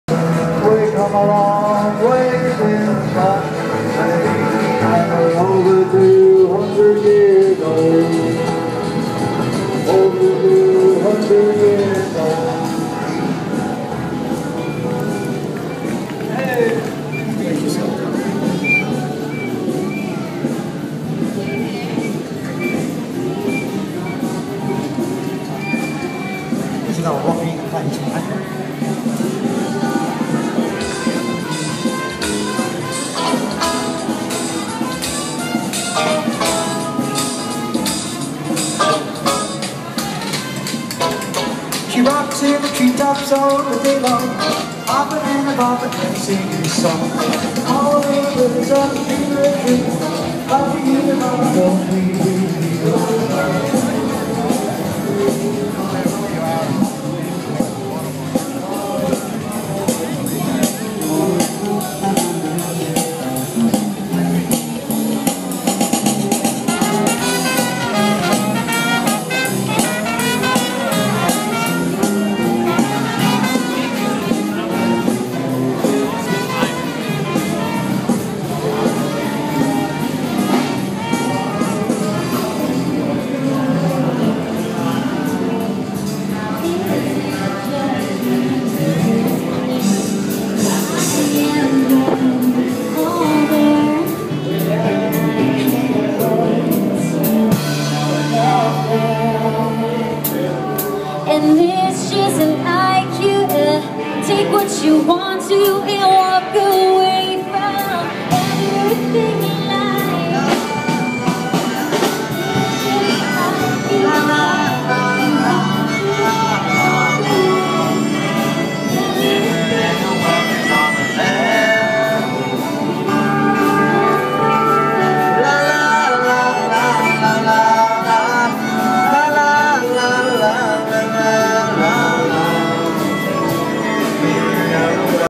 Tamworth. crazy Peel Street, in real time. Turn speakers up to distortion for real effect.
Turn your speakers right up to get some idea what it is like.